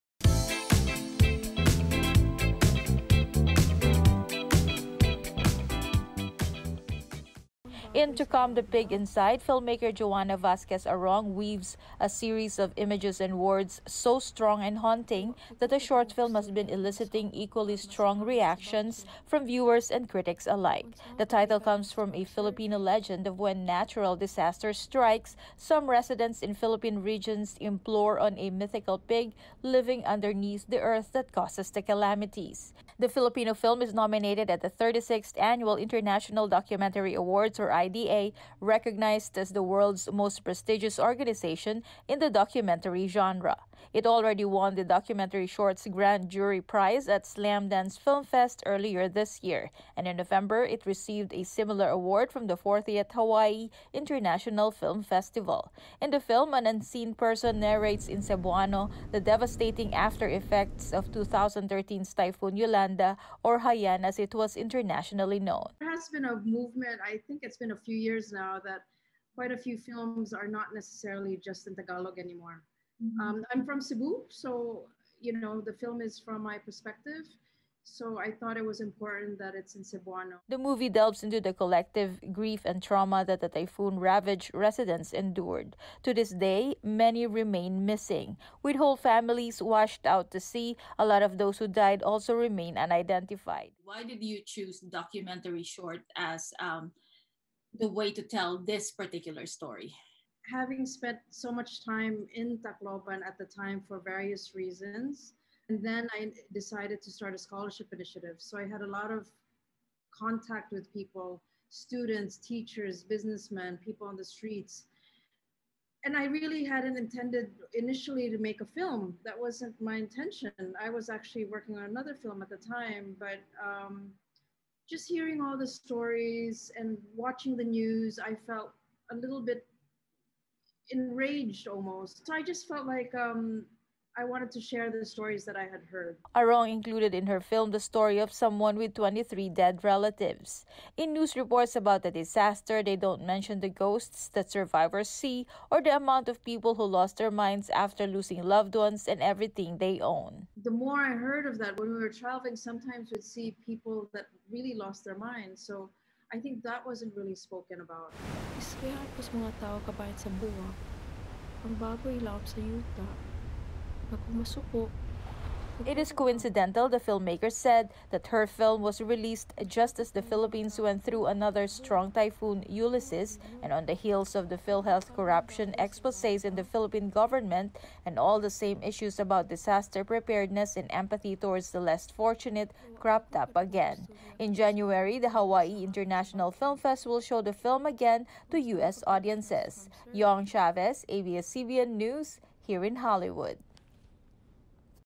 Cebuana filmmakers Typhoon Yolanda documentary nabs U.S. awards
Joanna Vasquez Arong's documentary short film weaves Filipino myths to understand how a small town copes with the devastation and trauma in the aftermath of a typhoon on an island in the Philippines. Yong Chavez speaks with the filmmaker who has been collecting awards in the U.S. this year.